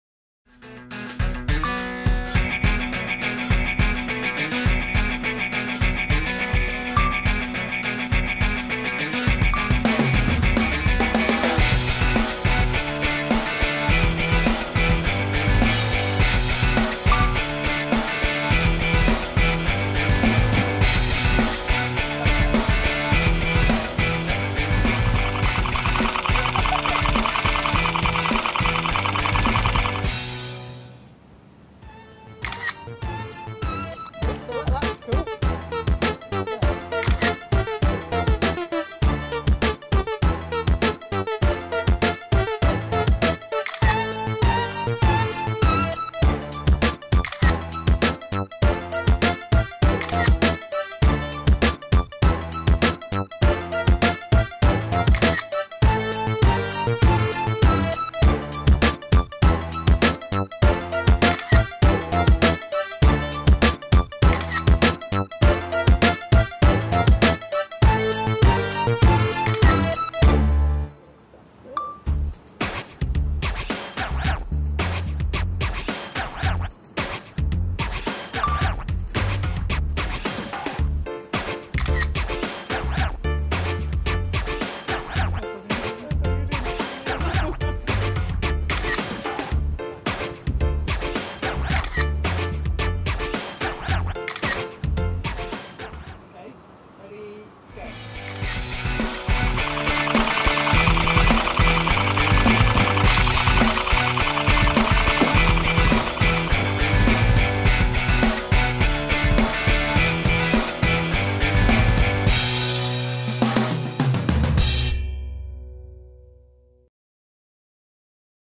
iPhone 5s Photo Shoot | Annabel Williams
Dare to leave your DSLR at home and try a whole photo shoot on your iPhone? I just tried this, and it was a lot of fun - however I also discovered a few things you need to look out for when using your phone for a "proper" full shoot, rather than just occasional snap shots on Instagram, etc. For lots of tips to help you with the pros and cons check out my blog